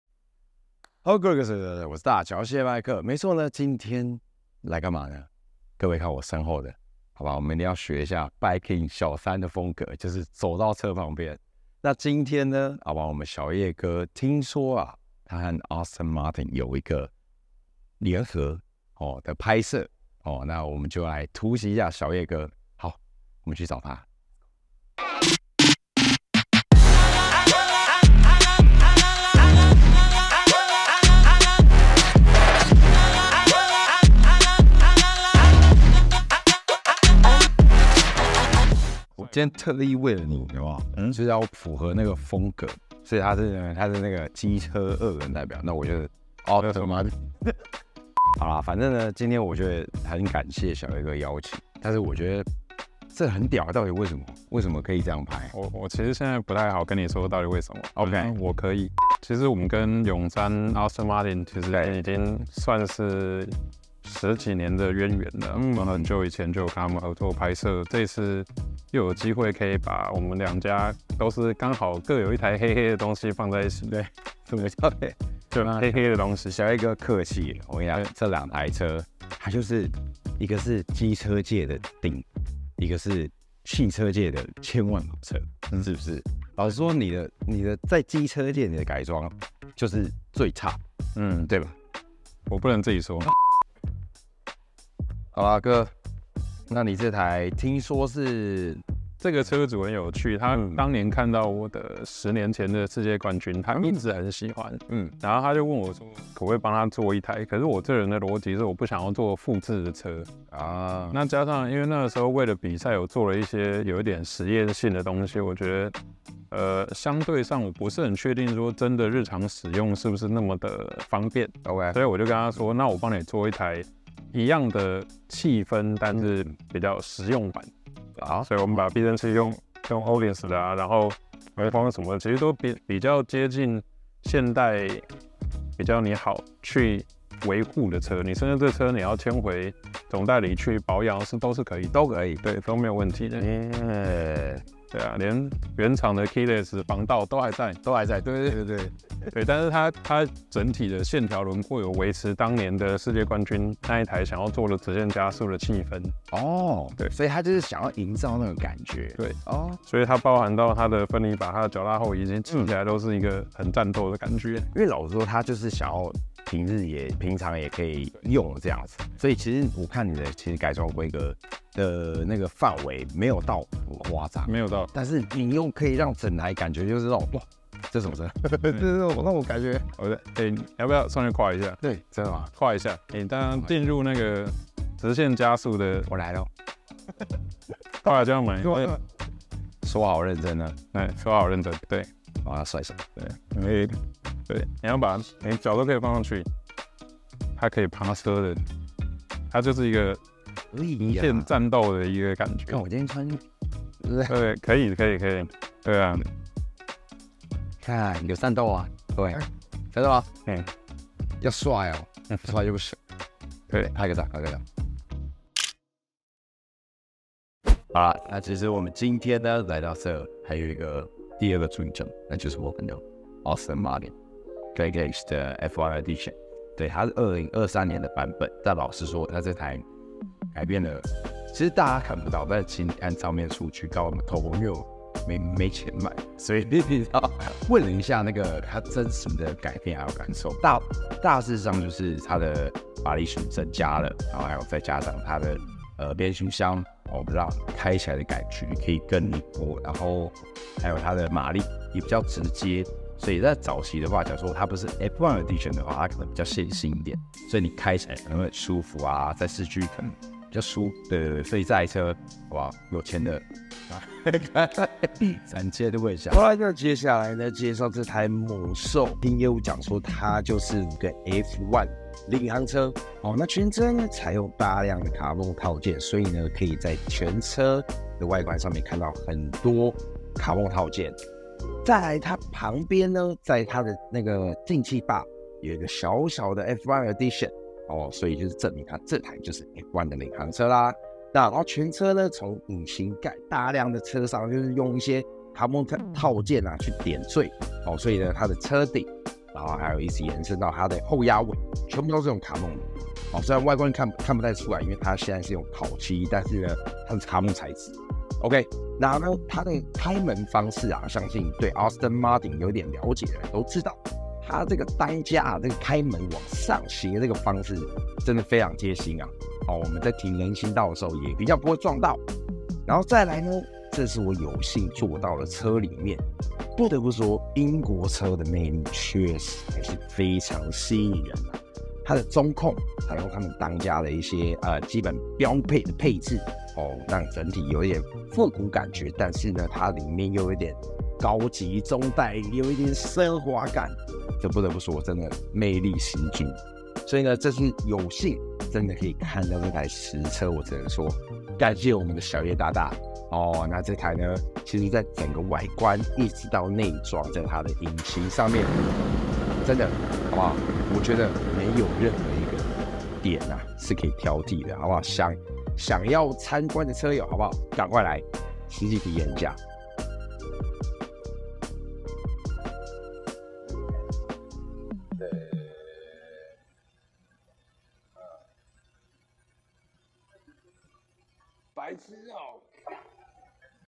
【麥克去哪裡？EP.1】當千萬級跑車遇上改裝車世界冠軍。｜阿斯頓馬丁｜Aston Martin Vantage F1 Edition｜大橋謝麥克 | Mike's Channel | 4K UHD
這次難得有機會可以突襲小葉哥的拍攝現場，看到這台F1特別版的Vantage，特地來記錄一下這次的經過。